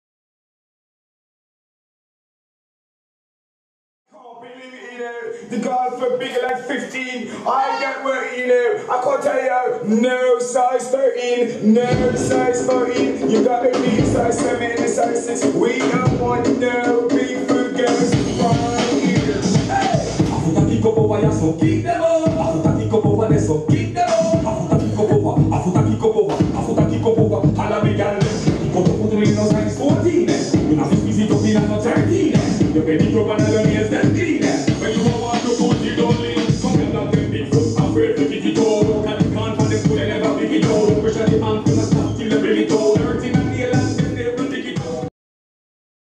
AC workshop 2014 (anniversary) - Alča Alyaschca (dancehall)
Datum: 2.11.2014
Speciální CHOREO workshopy pro začátečníky a mírně-pokročilé
Místo: AC loft, Olomouc, CZ
Lektor: Alča Alyaschca
FB: Alyaschca Crew, Alyaschca Crew - 10 YEARS ANNIVERSARY